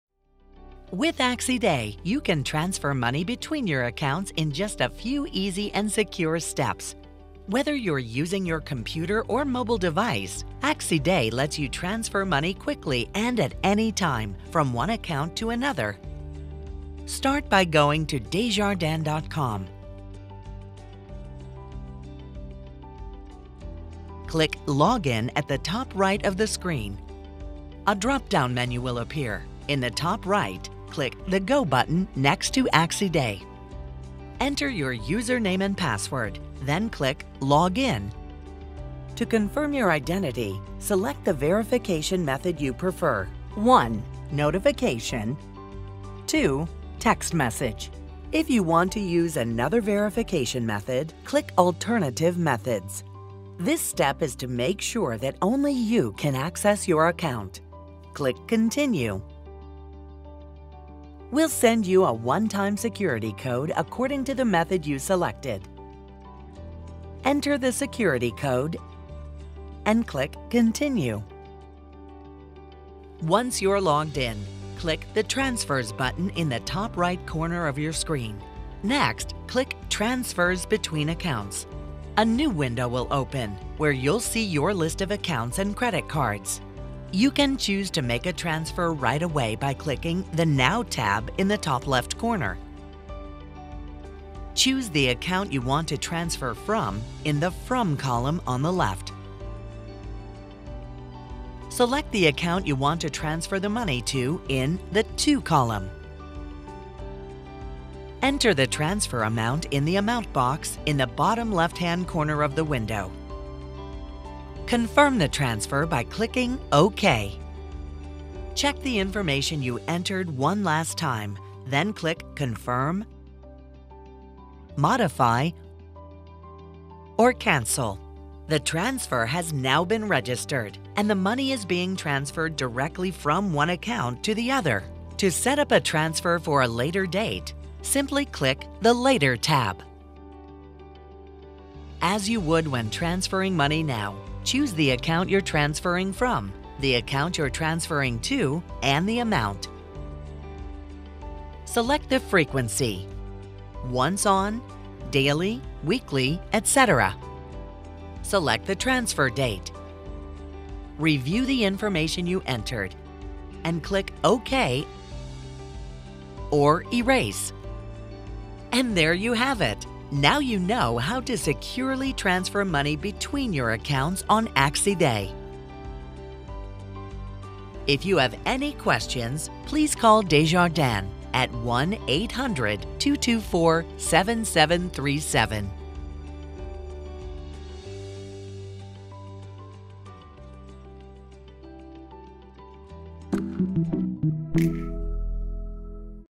Transferring money between your accounts on AccèsD
This tutorial shows you how to transfer money between your accounts on AccèsD online.